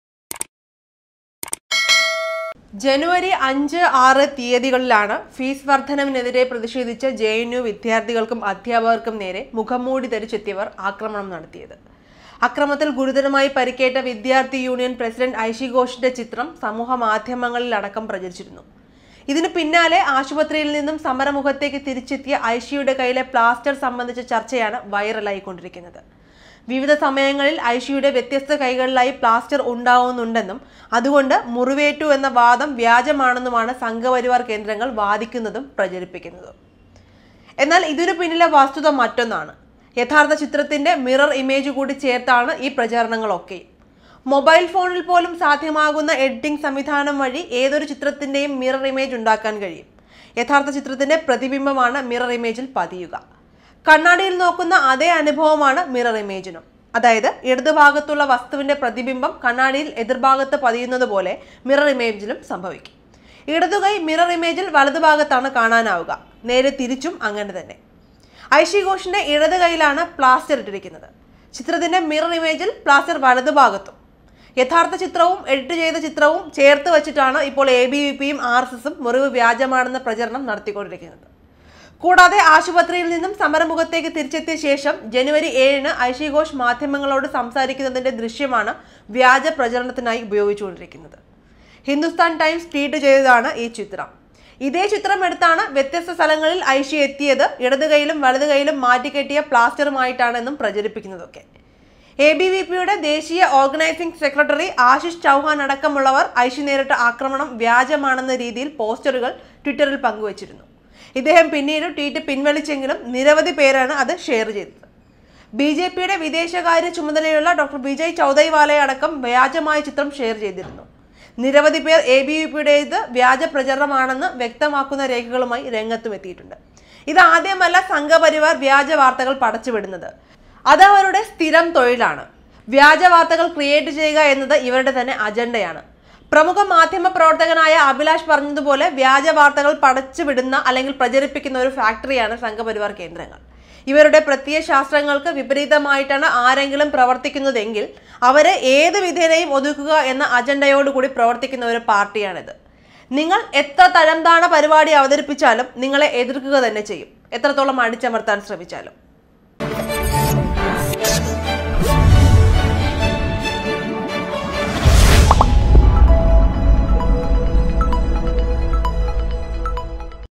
അയ്ഷി ഘോഷിന്റെ അപകടം വ്യാജമോ? ഇടതു കയ്യിലെ പ്ലാസ്റ്റര്‍ എങ്ങനെ വലതുകയ്യിലെത്തി?;
അയ്ഷി ഘോഷിന്റെ അപകടം വ്യാജമോ?
ഇടതു കയ്യിലെ പ്ലാസ്റ്റര്‍ എങ്ങനെ വലതുകയ്യിലെത്തി?;
Stay Updated with our youtube Channel :